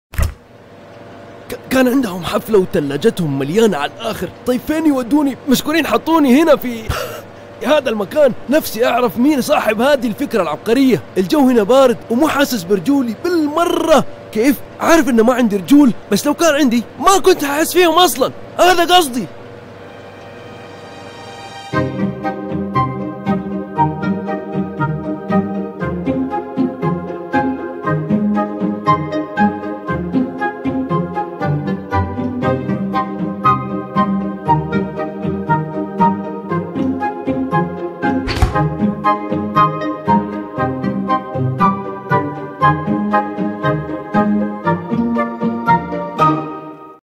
حبحب أبو دومة تجمد و ماحد سائل فيه
هذا اللي يحصل لما ما يكون في مساحة كافية، وما تقدر تحول الفريزر لثلاجة.

متوفرة في المملكة العربية السعودية. اكتشف المزيد من الرابط